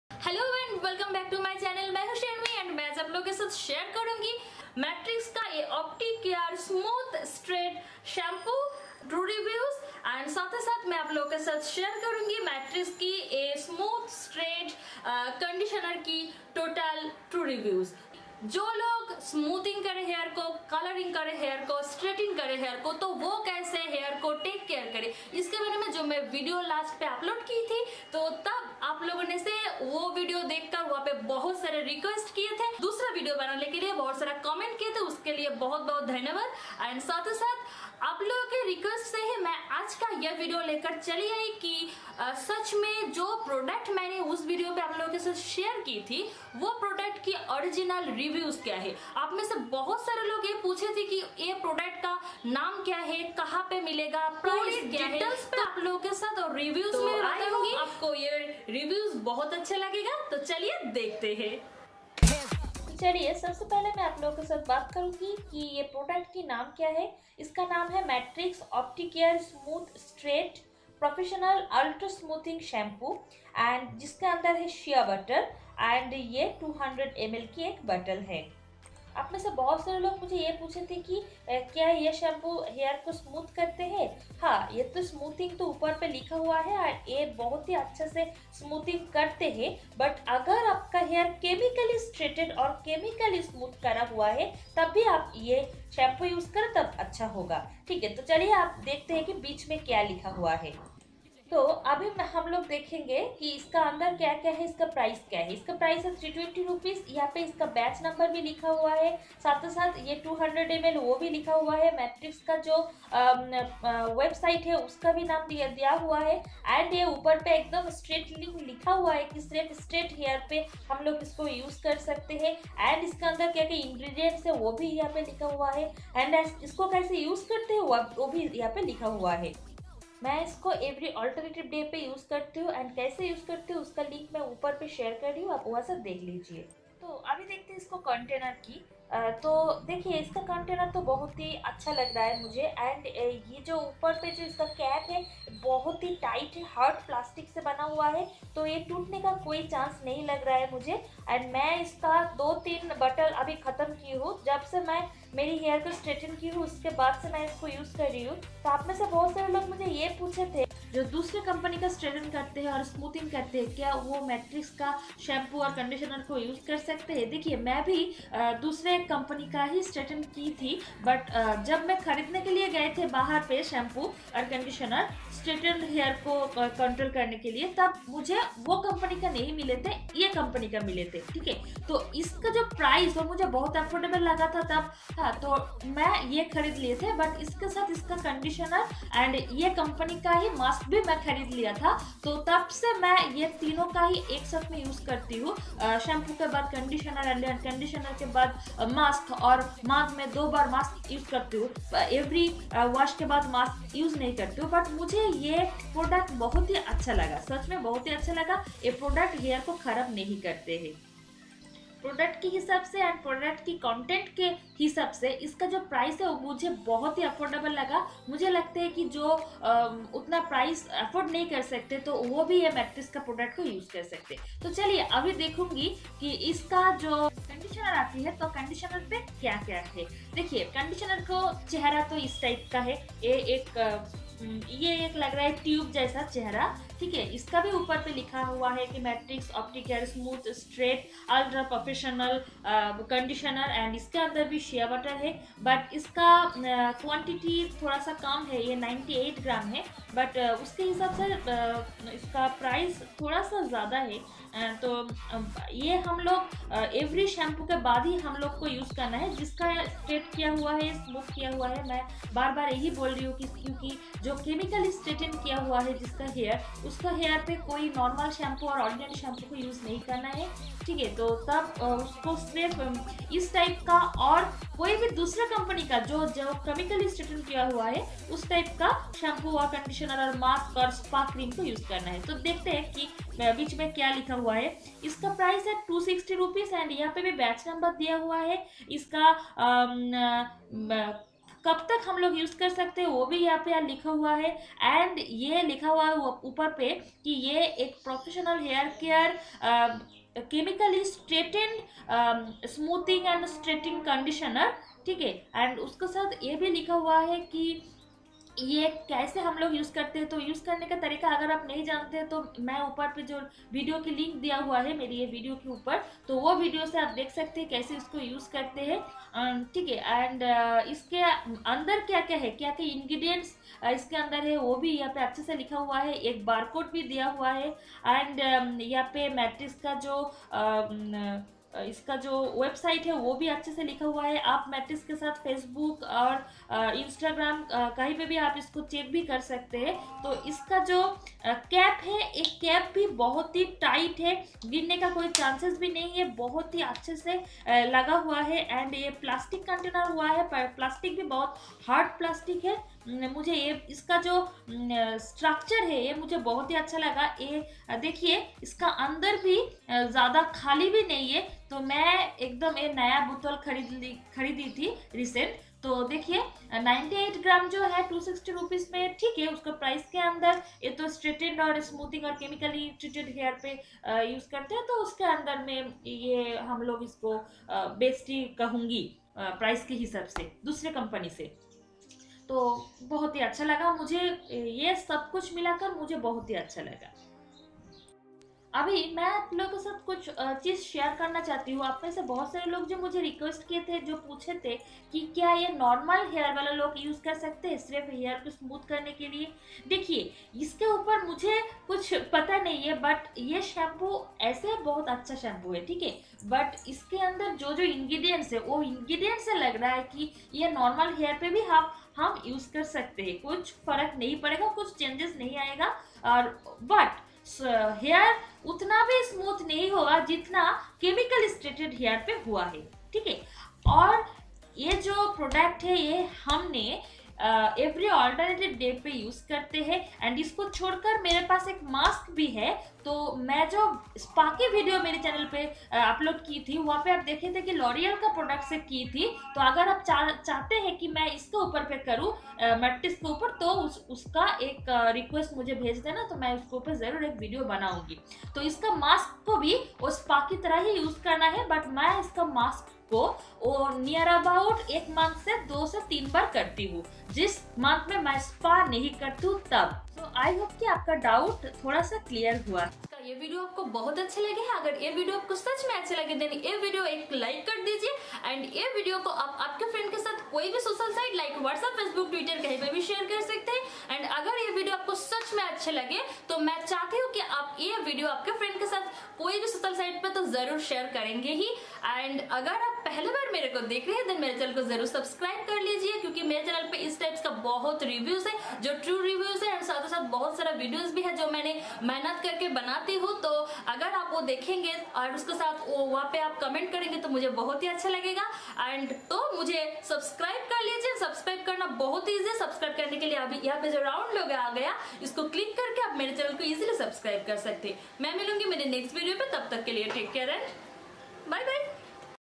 Matrix shampoo and conditioner for straight and smooth hair true reviews/moreglamours
Its a requested video about matrix straightning shampoo and conditioner's reviews..